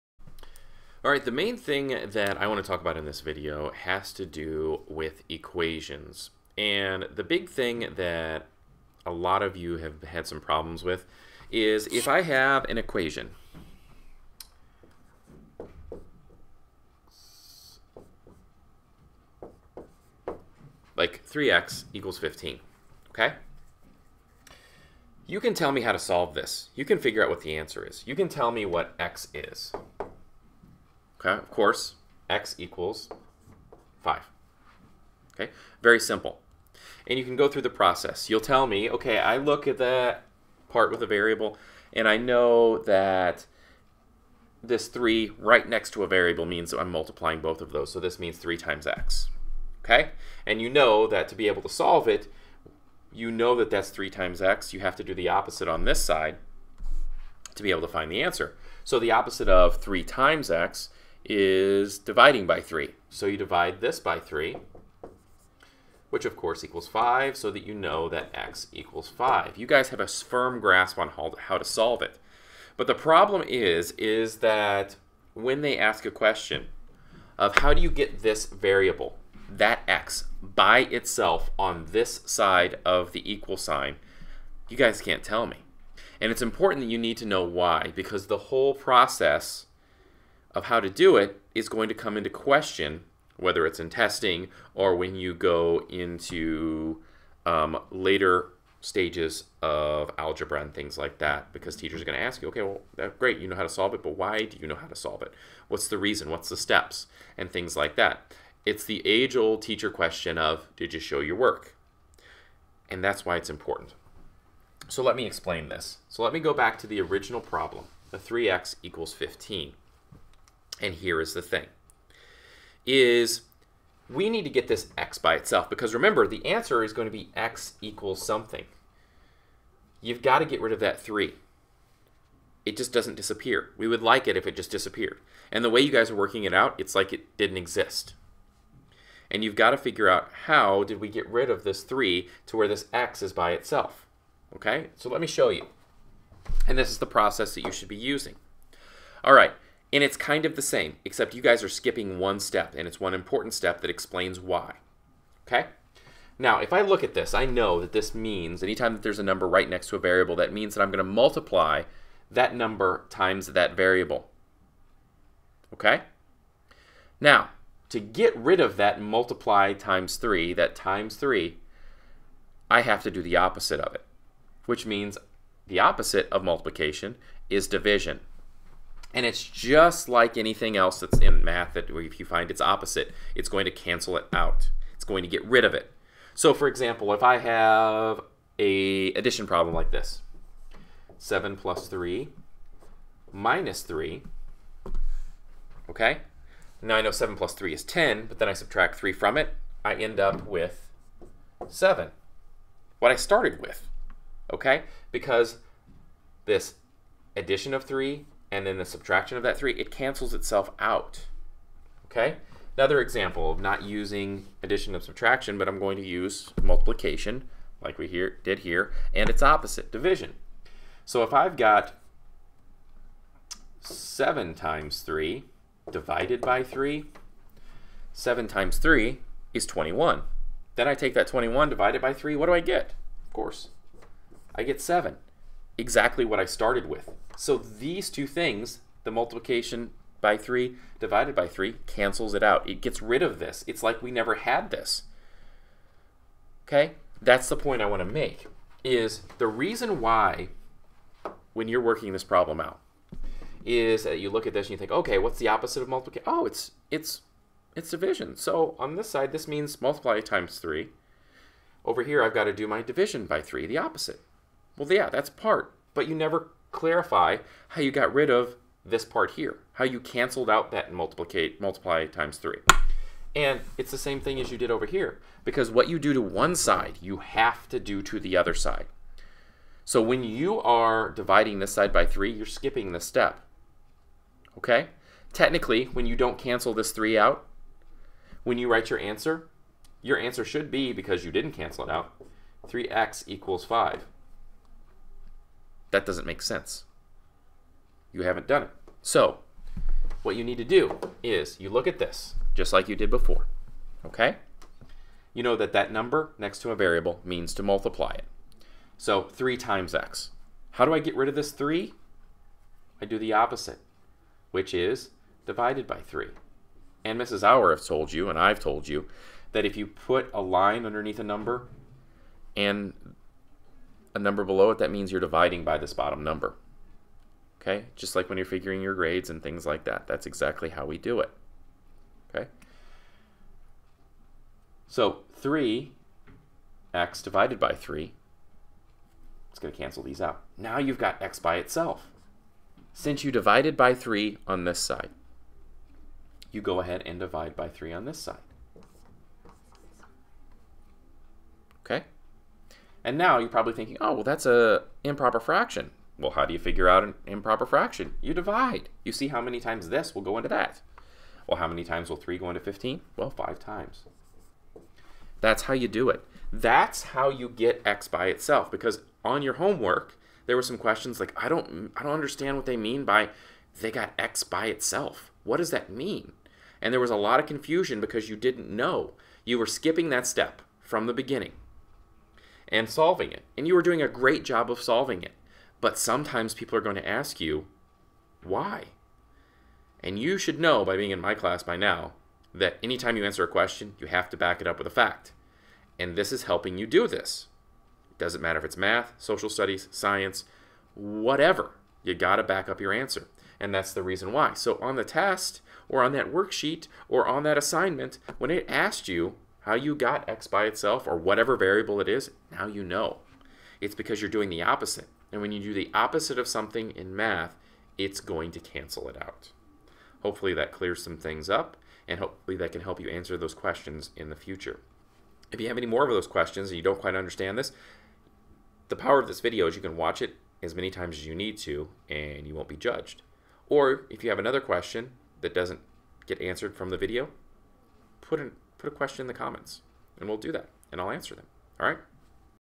Solving Equations
This tries to answer the question, "What do they mean how did I get the variable by itself?"